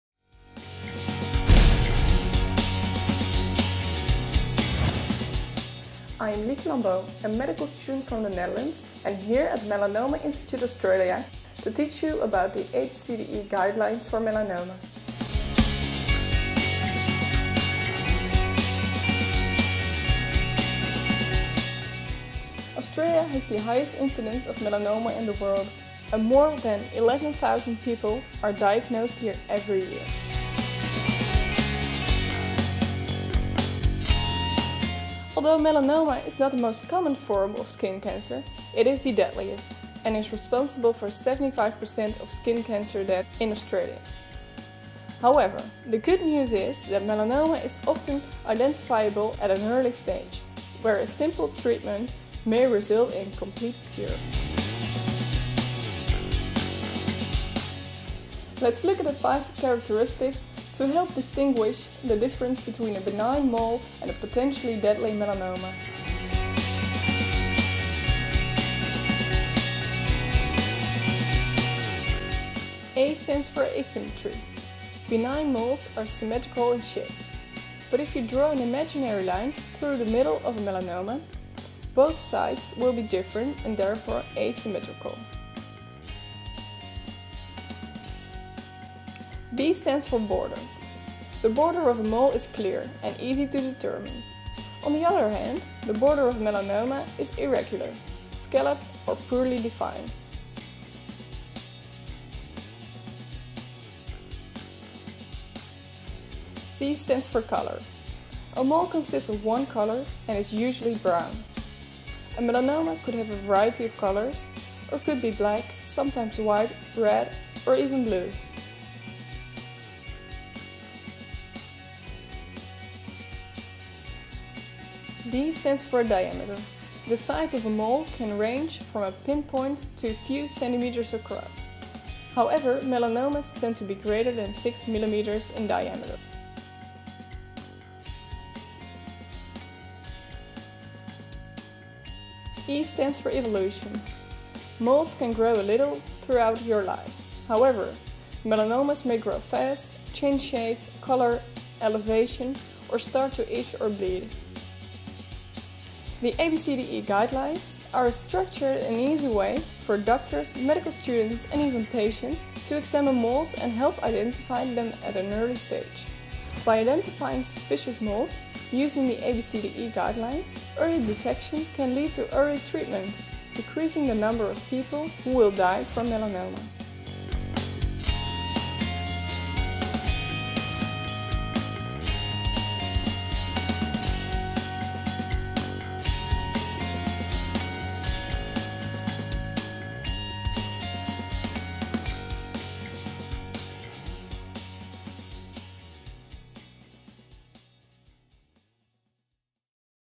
ABCDEs of Melanoma
By identifying suspicious moles using the ABCDE guidelines, early detection of melanomas can lead to early treatment.
Australia has the highest incidence of melanoma in the world, and more than 11,000 people are diagnosed every year. However, the good news is that melanoma is often identifiable at an early stage where simple treatment may result in a complete cure.
This video was produced by international medical students at Melanoma Institute Australia in 2012.